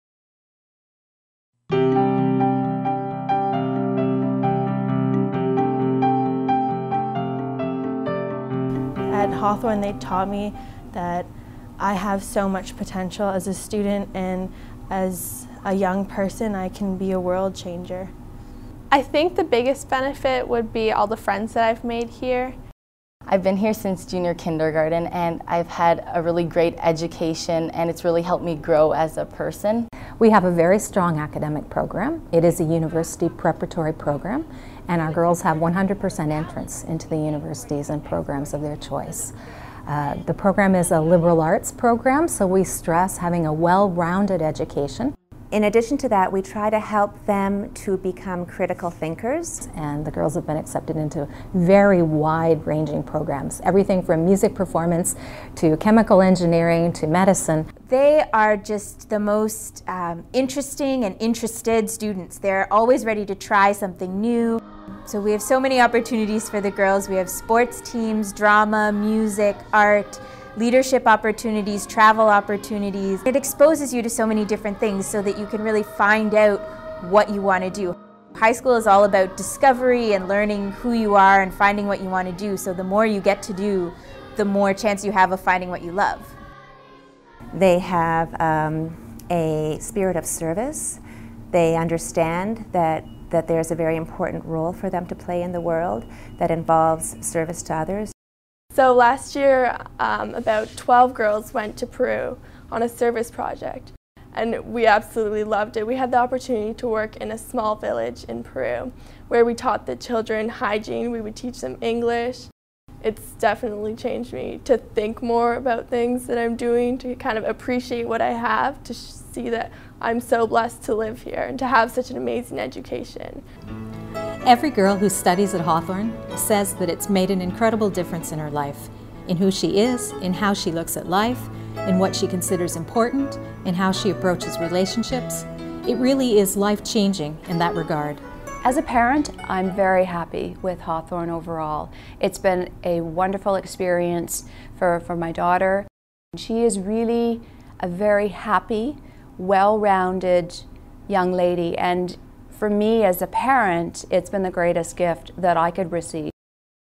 Hawthorn School for Girls Upper School
A glimpse of Hawthorn School for Girls' Upper School.